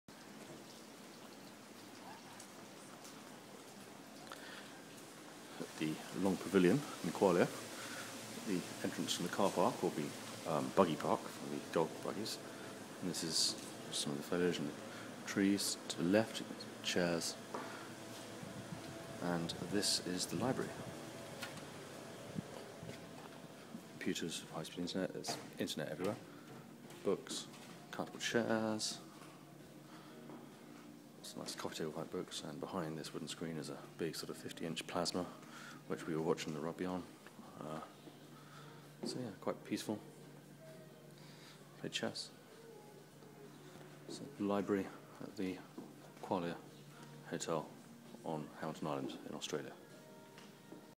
Library at the Qualia Hotel, Hamilton Island, Australia
Walking into the Library from the buggy park, in the long pavilion at the Qualia Hotel, Hamilton in the Whitsunday Islands, Australia